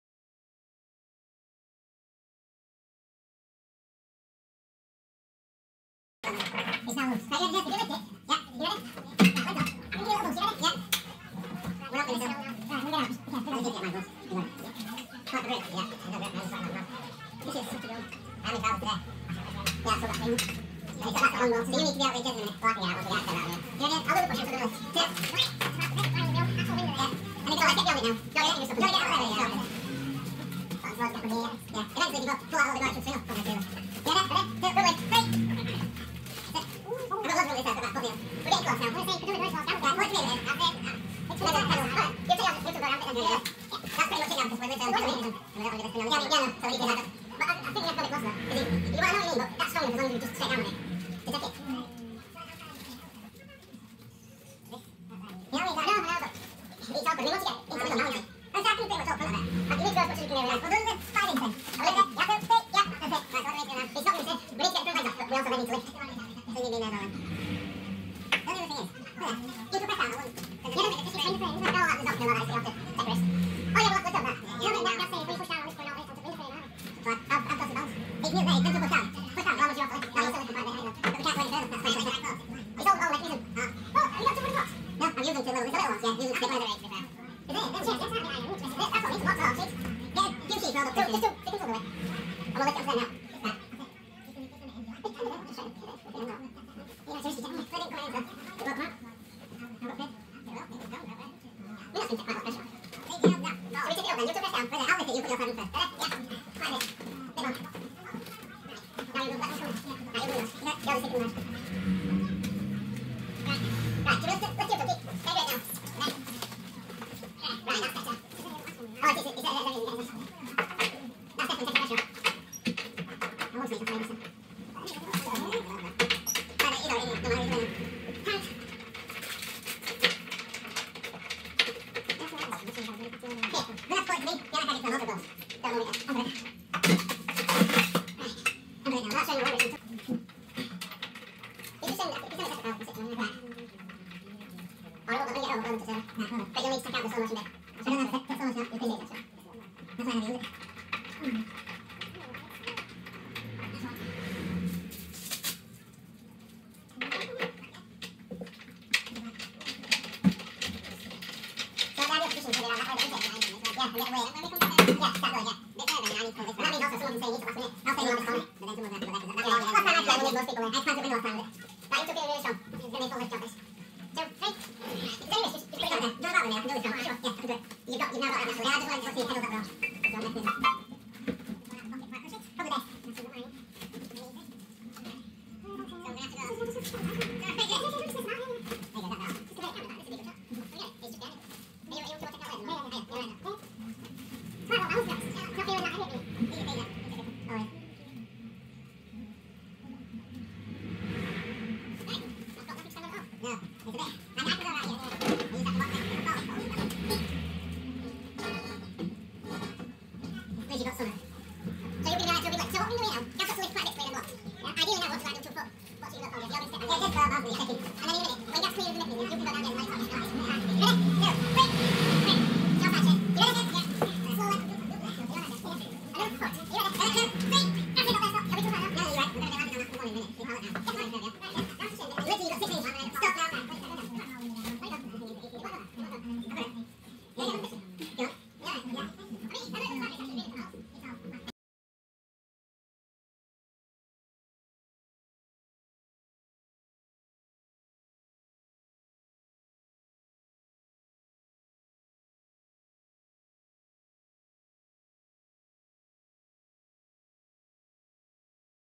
Moving a Yamaha S4 Grand Piano Through A Small Window.
Sorry for the terrible quality, but here we are moving a Yamaha S4 grand piano through a very small window, in a cottage in Lindley, Huddersfield, UK.